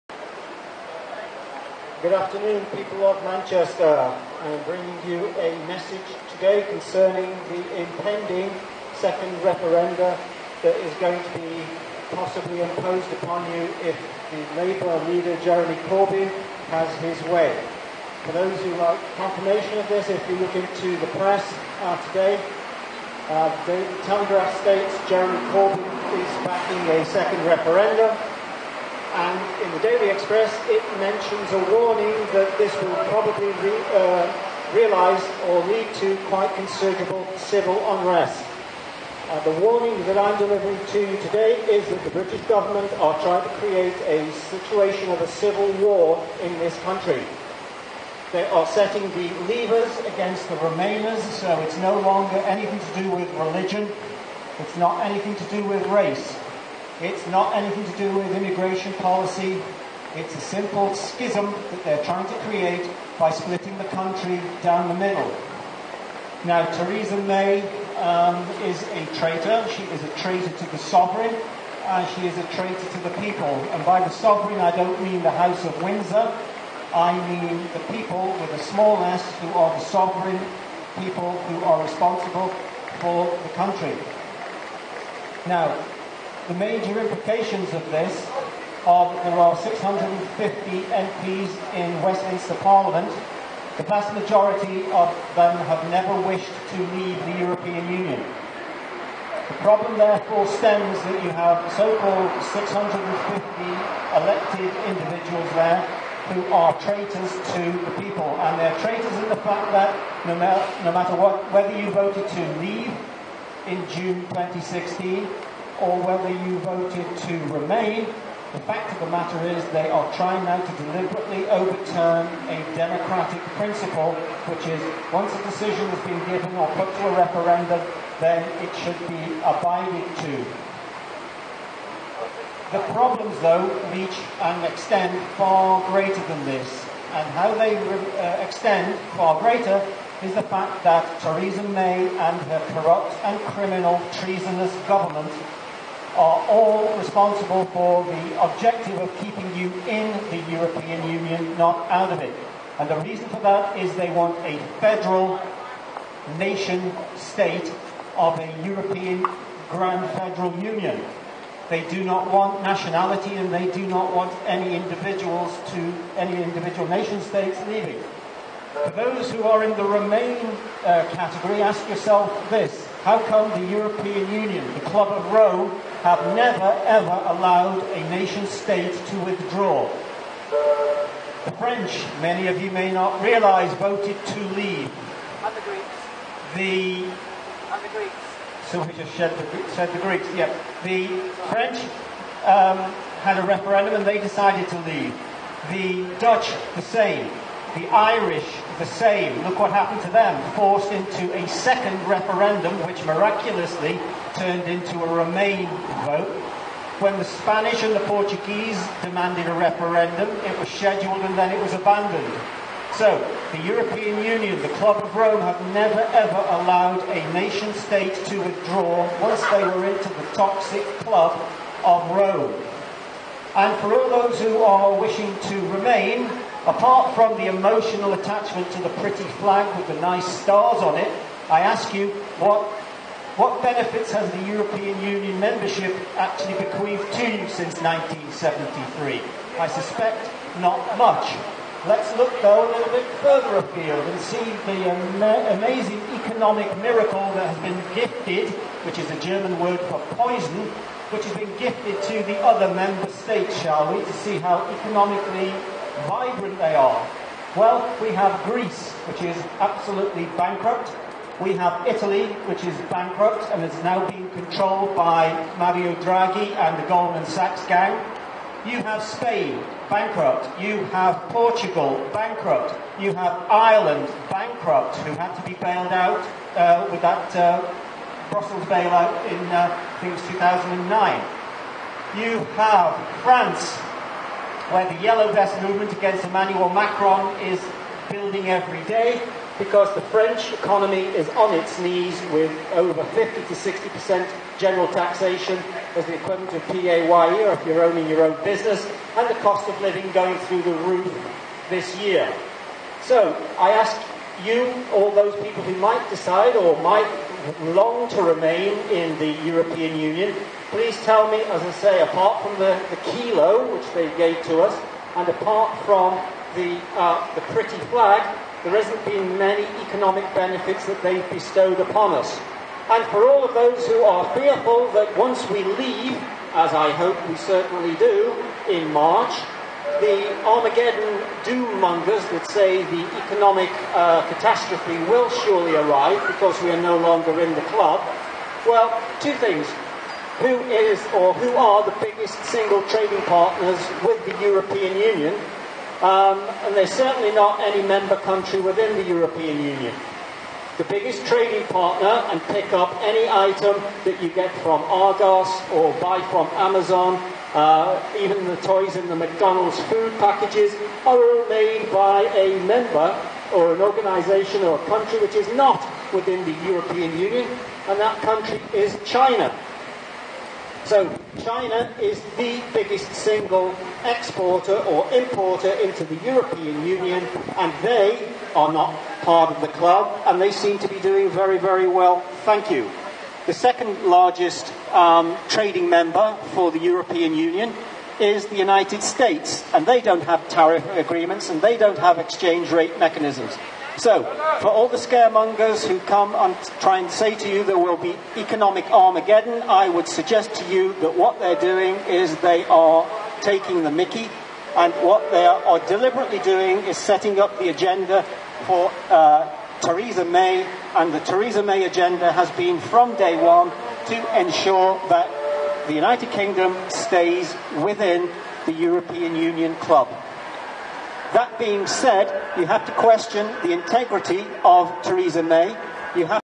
(1) Peter of England - The ReTaking of England begins
Public Speaking in Manchester Piccadilly Gardens.

The Corbyn-May plan is to create civil unrest and turn everyone and anyone against their "imaginary" opponent.
There never was an intention to let the British leave the EU and the proof is before your very eyes with Corbyn "the traitor" now openly calling for a 2nd Referendum to decide that which already has been decided. If they allow this then their will be BLOOD ON THE STREETS...unless Peter of England is listened to and allowed to guide the process. Want change? He will deliver it BUT you need to back him up.

Had enough? Then get in touch and JOIN us now.

"If not for this, then for what?"

BREXIT2EXIT - Make England Great Again Website -
EXITEERS WANTED